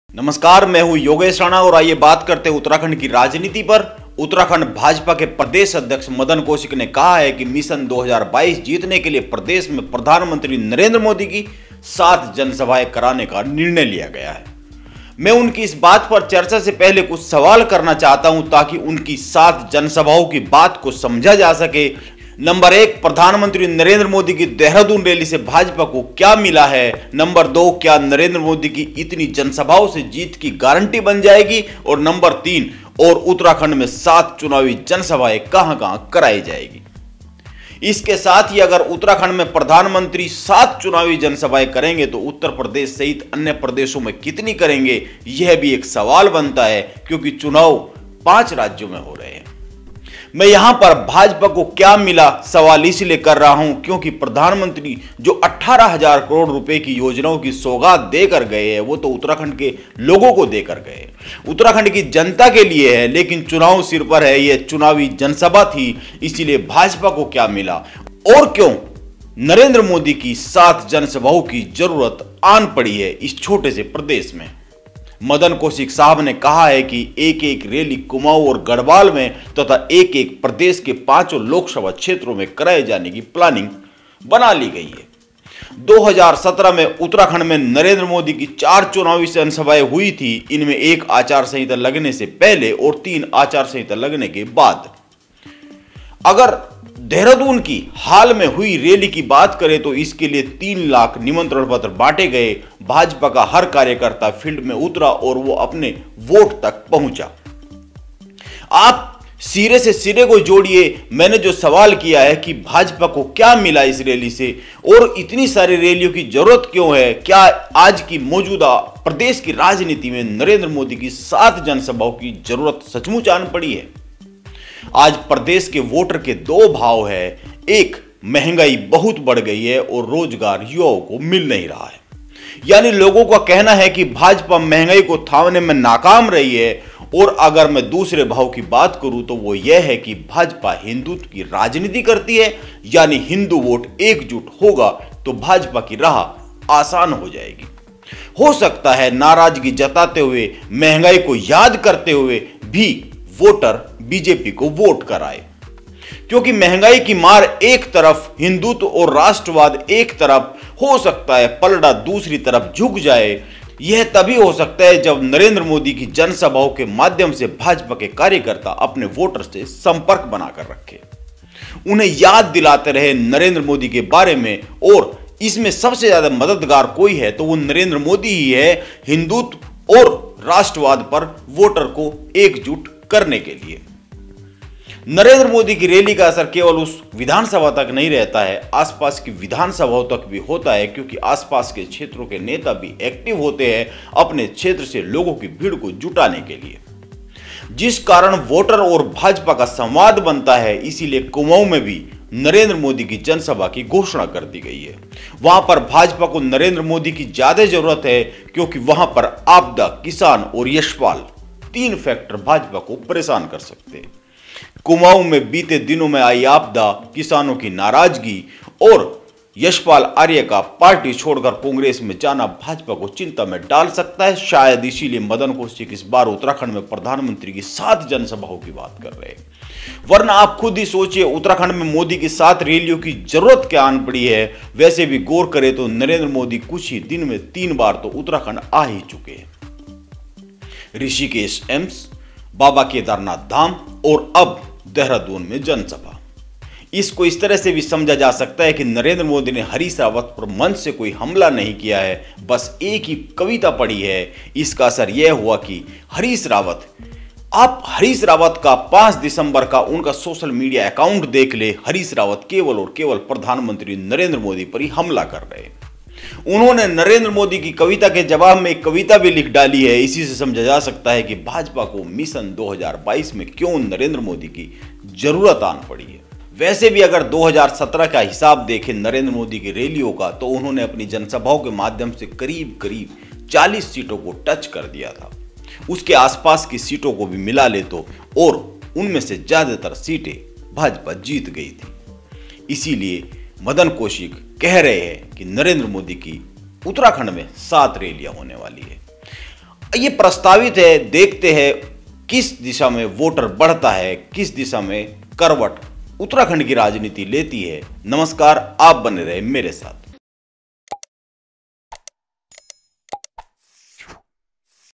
मोदी की रैली से उत्तराखंड भाजपा को क्या मिला? ​| Dehradun | Yogesh Rana
What did Uttarakhand BJP get from Modi's rally? | Dehradun | Yogesh Rana

नरेंद्र मोदी की रैली से उत्तराखंड भाजपा को क्या मिला? ​| Dehradun | Yogesh Rana

Narendra Modi,
Dehradun,
Pushkar Singh Dhami,
uttarakhand election,
Narendra Modi's public meetings,
uttarakhand 2022 opinion poll,